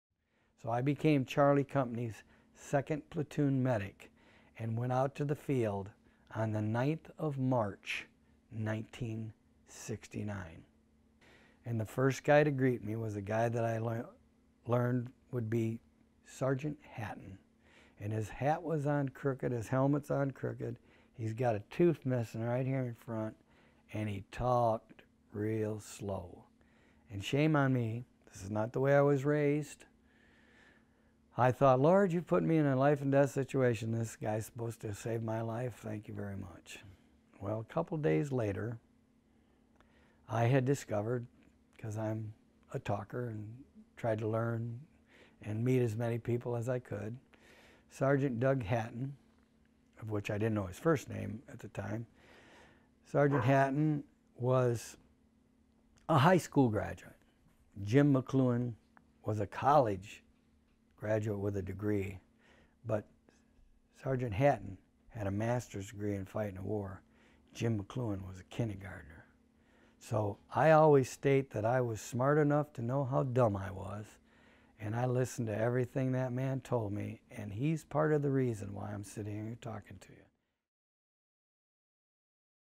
Sgt. Doug Hatten: Master’s degree in fighting a war
James McCloughan reminisce about  Doug Hatten, a man he credits to have kept him alive in Vietnam.